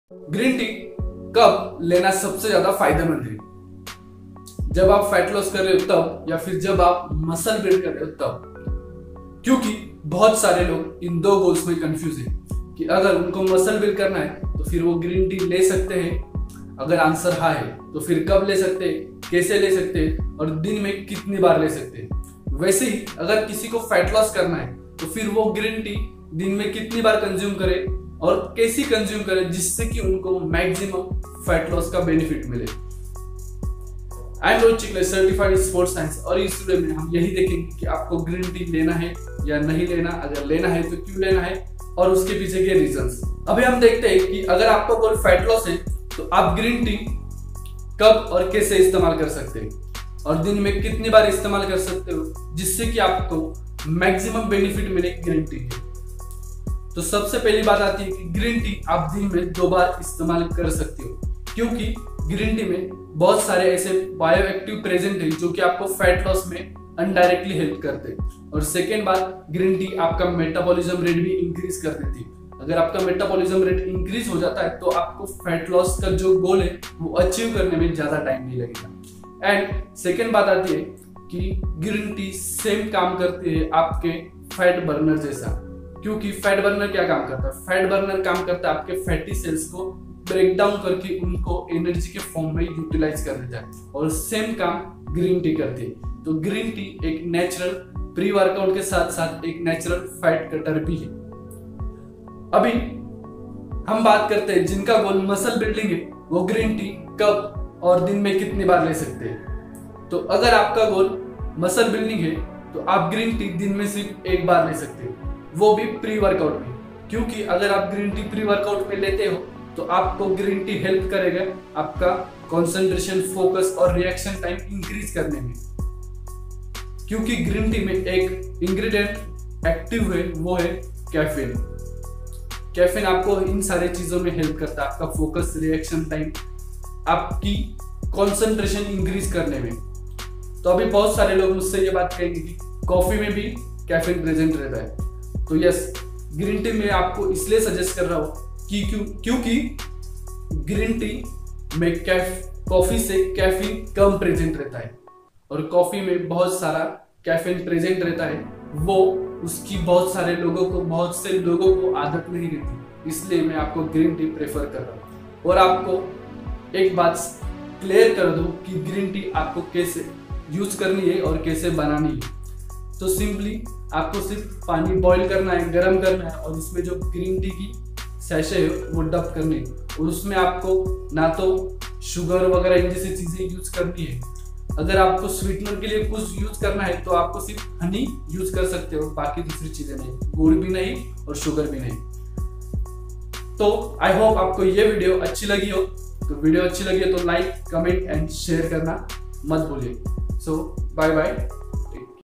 How to Use Green Tea🍵 For Muscle Building & Fat Loss || ग्रीन टी कैसे ले जिसे की, अच्छे रिज़ल्ट आए।
App ko agr muscle build karna he ya fir fat loss. To fir app green tea ko kis tarha se use kare? Jise ki jyada fayda ho sake. Ye hum is video me dekhenge.

•    Please take care🤗, stay positive and find some time for having fun and smiling.